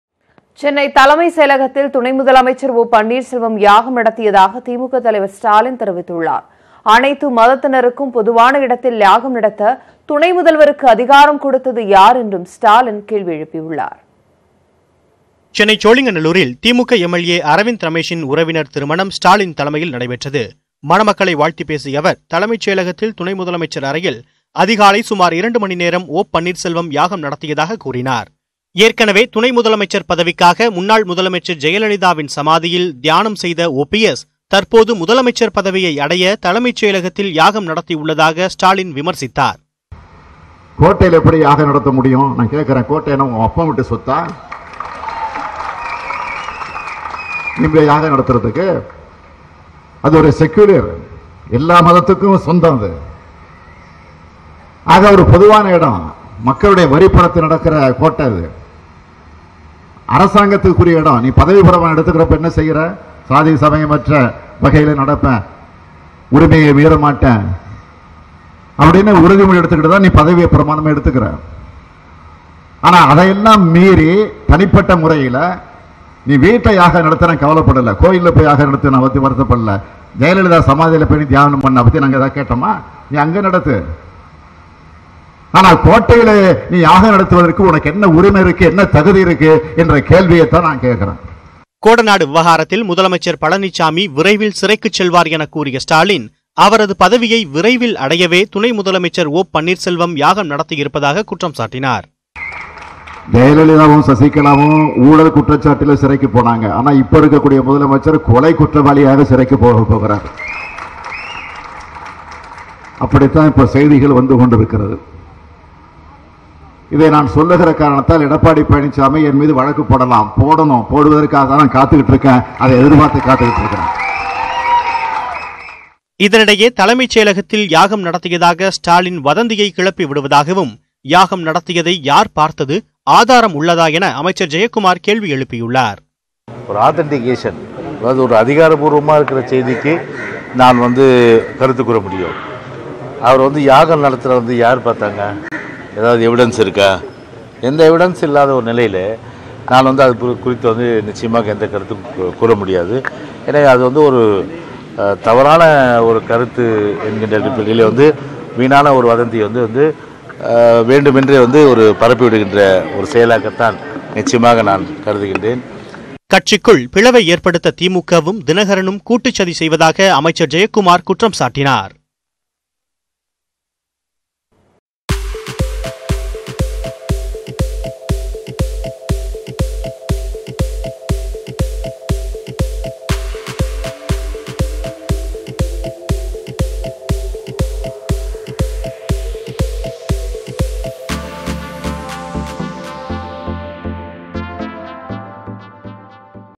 துணை முதல்வர் ஓபிஎஸ்-க்கு சரமாரி கேள்வி | ஓ.பி.எஸ் பதில் அளித்தே தீர வேண்டும் - மு.க.ஸ்டாலின்
சென்னை தலைமை செயலகத்தில் துணை முதலமைச்சர் ஓ.பன்னீர்செல்வம் யாகம் நடத்தியதாக திமுக தலைவர் மு.க.ஸ்டாலின் தெரிவித்துள்ளார்